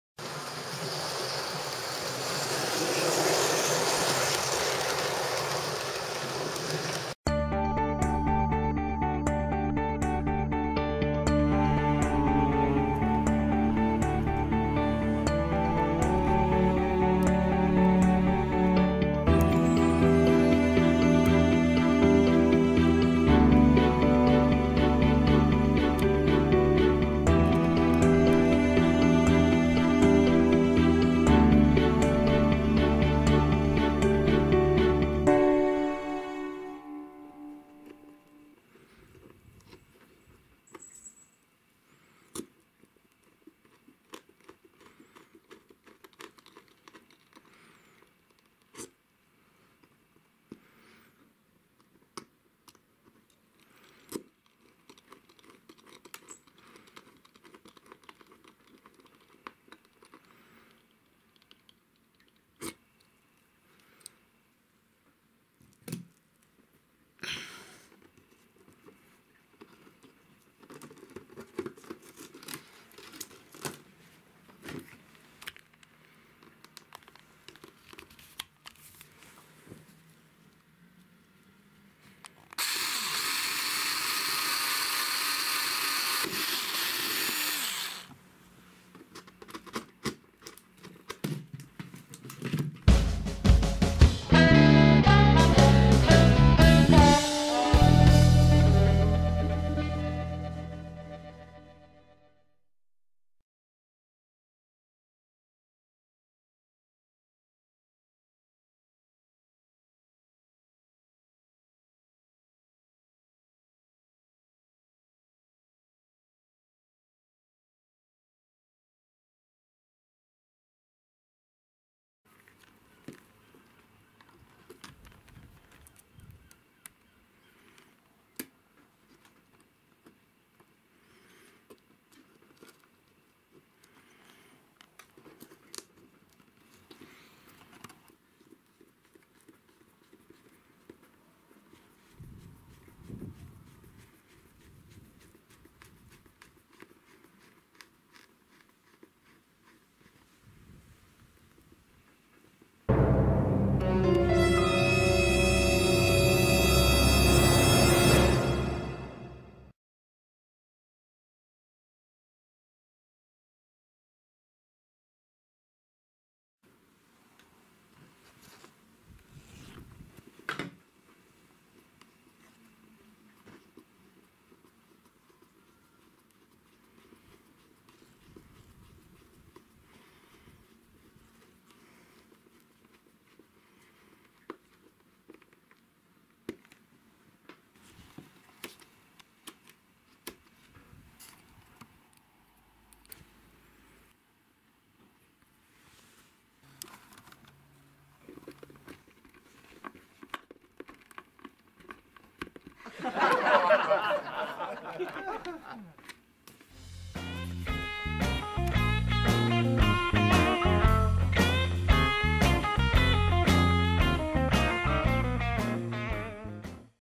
Fitting a PECO PL-11 point motor to a Hornby Trakmat Layout.
Search for "10mm OO gauge track screws" on eBay to get the screws and screwdriver shown in this video. The screws are self tapping but they are liable to go in at an angle if you do not first drill a hole as illustrated in this video.
Any questions? Please post them on our Facebook Group.